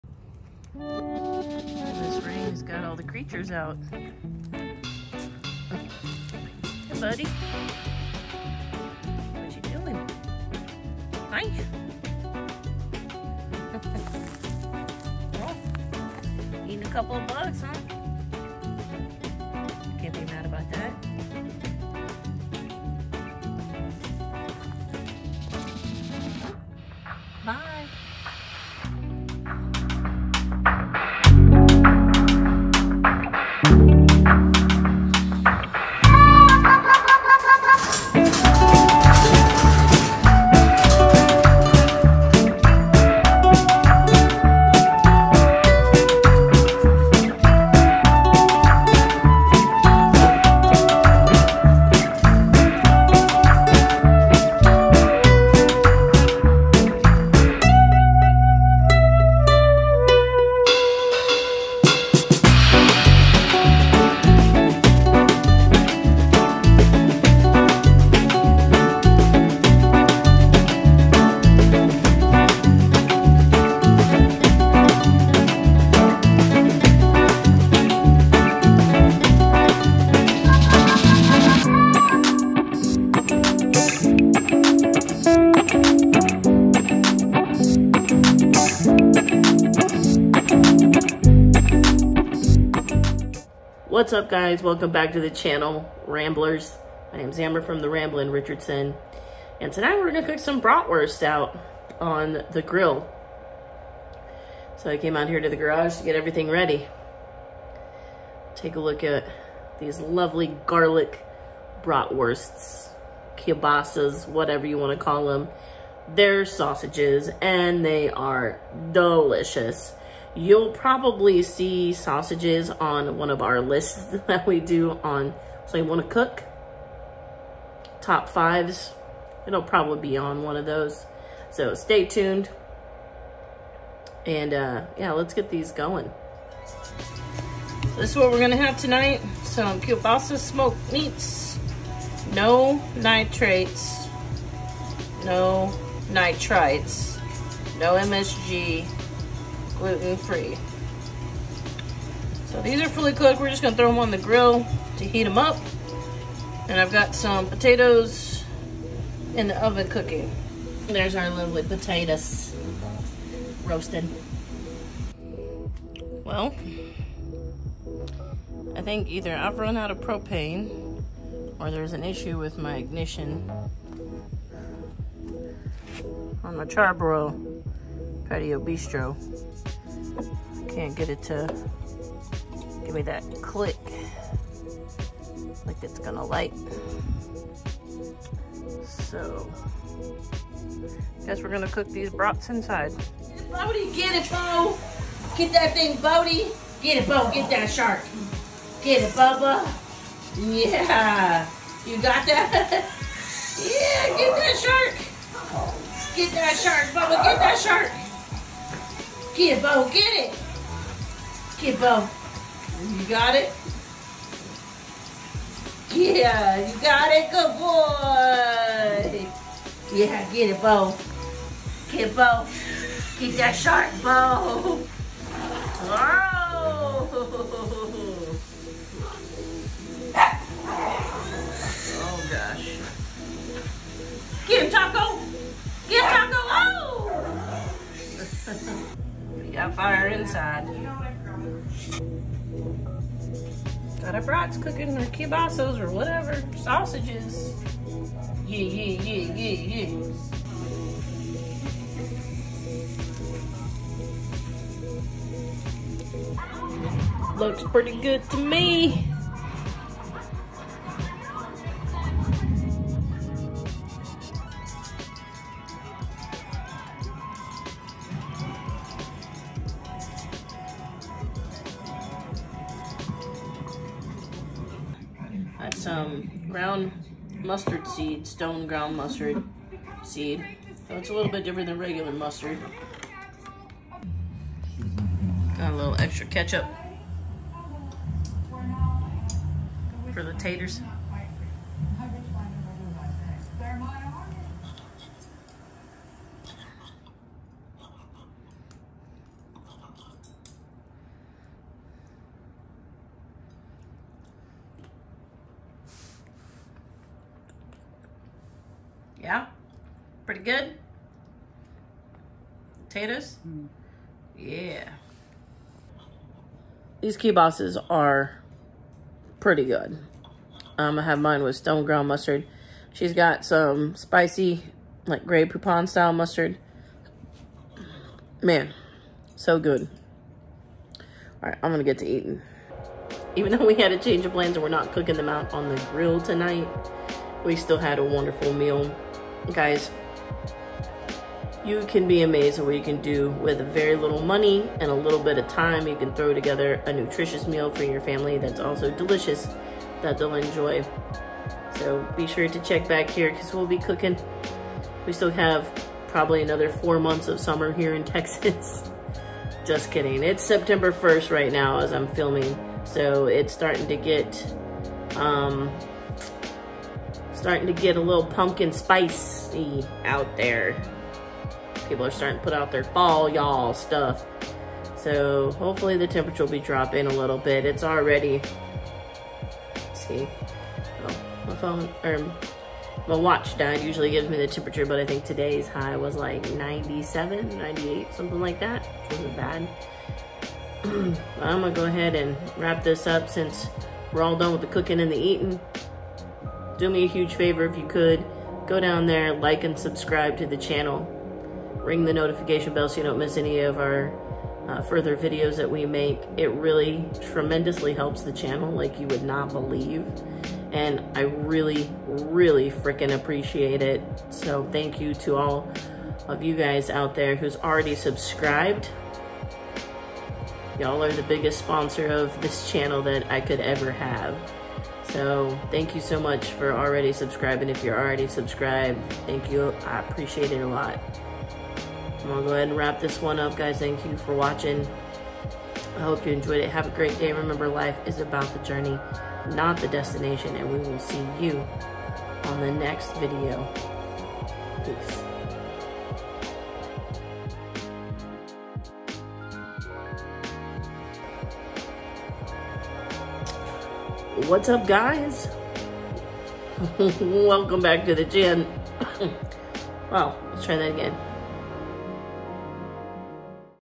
Cheap and easy? We have a problem!
It is a sad day!  So my propane @charbroil Patio Bistro isn't lighting.  I think either my ignition switch is broke or I'm out of propane.  So we will have to use a pan to cook our sausages! I'll have to figure out what is wrong with the grill because I still have lots of plans for it this season.  Otherwise, our dinner tonight was a hit.  Garlic sausages and roasted potatoes.  A cheap and easy dinner that takes literally no time at all to fix.